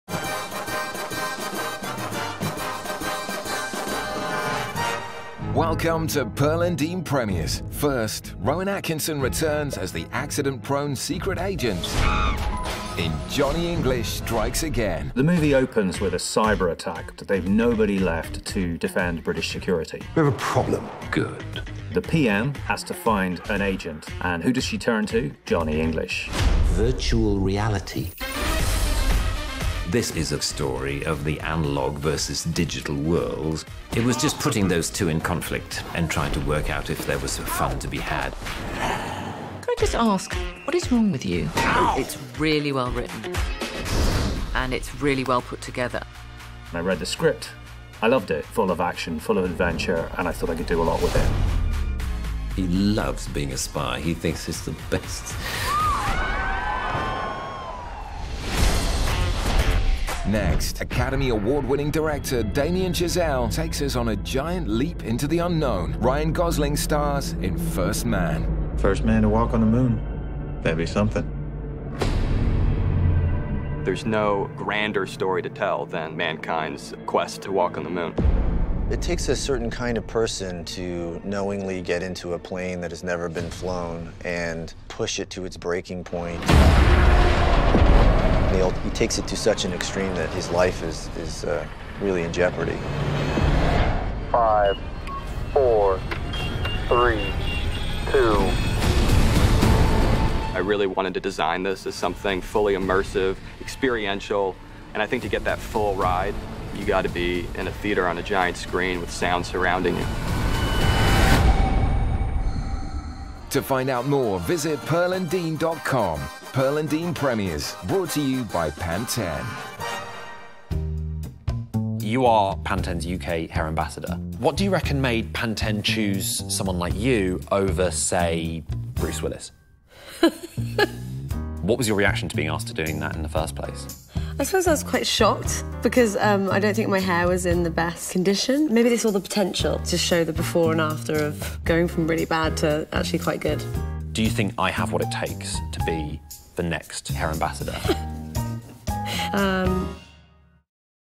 Pearl & Dean Premieres Episode 7: Johnny English Strikes Again and First Man
In the latest episode of Pearl & Dean Premieres, we speak to Johnny English Strikes Again director David Kerr and stars Rowan Atkinson and Emma Thompson about what to expect from the new spy comedy and why it's the best instalment yet.

Then we chat to the Damien Chazelle and Ryan Gosling, director and star of First Man, which recounts the story of Neil Armstrong's attempts to be the first man to set foot on the moon.